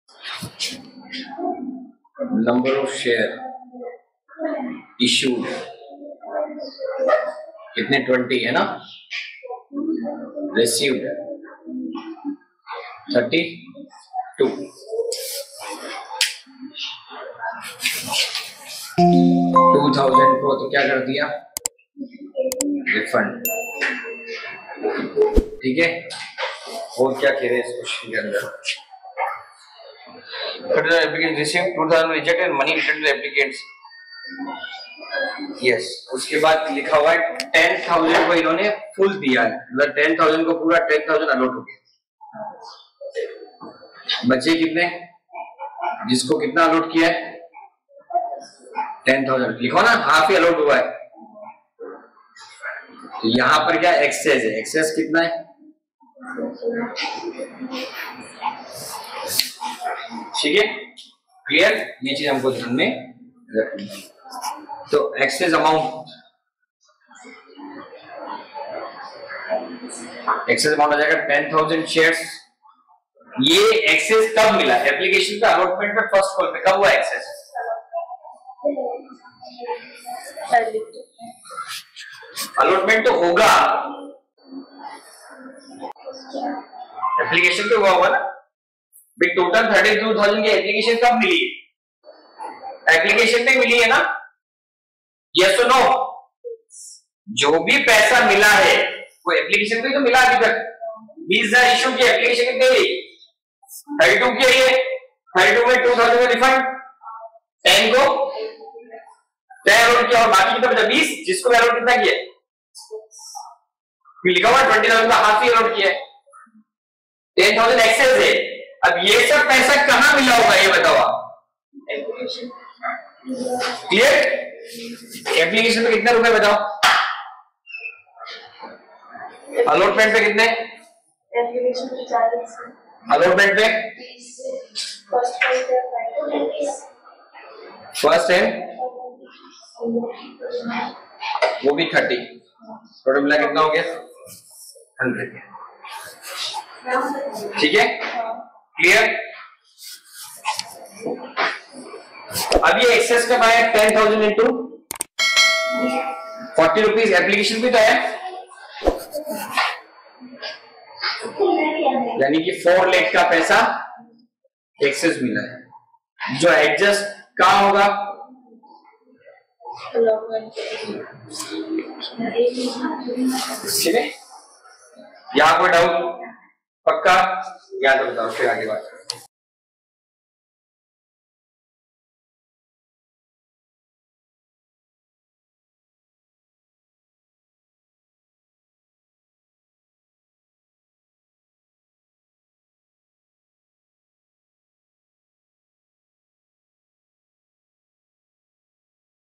Understanding Company Accounts: Issuing Shares (Class 12 NCERT) Lecture-28
Welcome to Success Coaching Centre! In this video, we delve into the Class 12th NCERT Accountancy syllabus to help you understand the complexities of Company Accounts, specifically focusing on the issue of shares. Our 28th lecture is designed to provide you with comprehensive knowledge, breaking down complex concepts into easy-to-understand explanations.

In this lecture, we will cover the intricacies of issuing shares in a company's accounts, offering a detailed insight into the principles and practices involved. We avoid jargon and focus on making this topic easily accessible to all students.

This video is a valuable resource for Class 12 students preparing for their Accountancy exams and anyone else interested in the subject. So, if you're looking for clarity on this subject, you're in the right place.